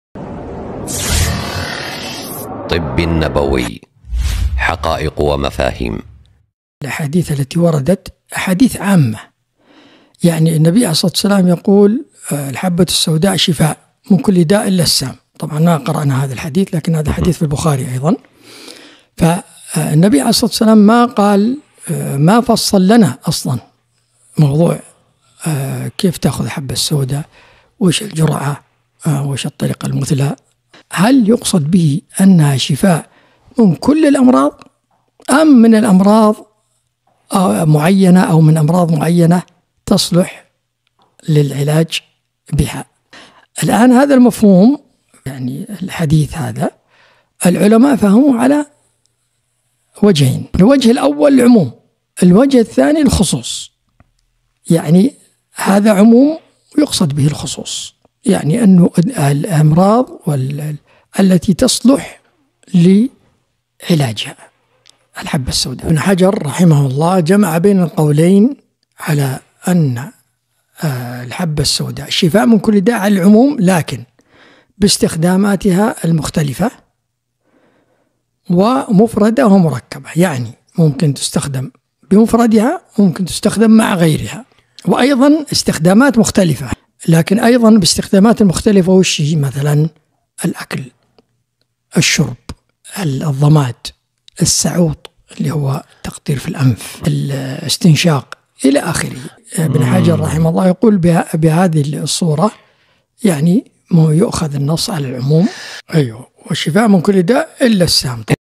معنى (شفاء من كل داء) الوارده في الحديث النبوي
أ. د. عبدالله باموسى